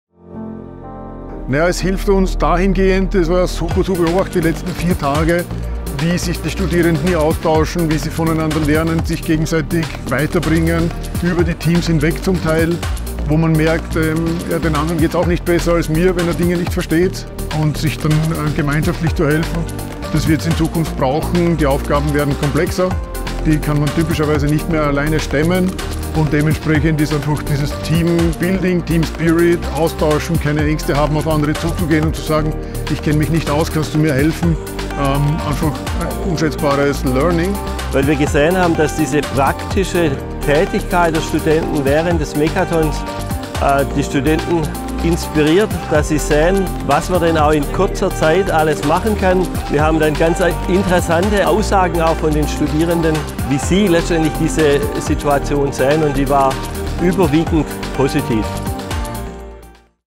Smart Green Island Makeathon – What are the benefits for students?
👉🏻 The MAKEATHON is a multicultural Innovation Festival for students from all over the world. ✨🌍

We asked professors why you should take part in our MAKEATHON 2024:

1. Work in a team 👩🏻‍🎓👨🏻‍🎓
2. Exchange ideas 💡
3. Learn from each other and help each other 📗
4. Approach other people and welcome everybody 🤗
5. Boost your career with a great project work 🚀
6. Be self-confident and strengthen others 🦾